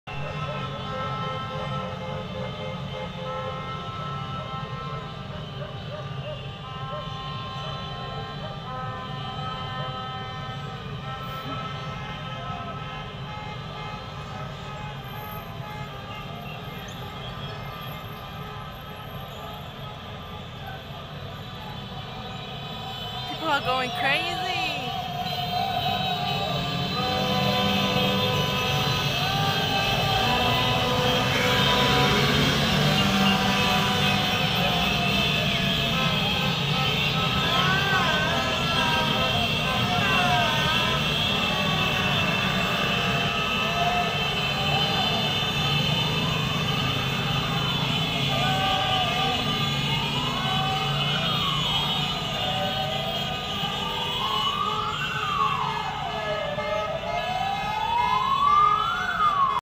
إحتفالات المراكشيين بالنصر والتأهل لنصف النهاية.
فوز المنتخب المغربي🇲🇦 على نظيره البرتغالي🇵🇹إحتفالات الفوز على البرتغال#تأهل المغرب لنصف نهاية كأس العالم#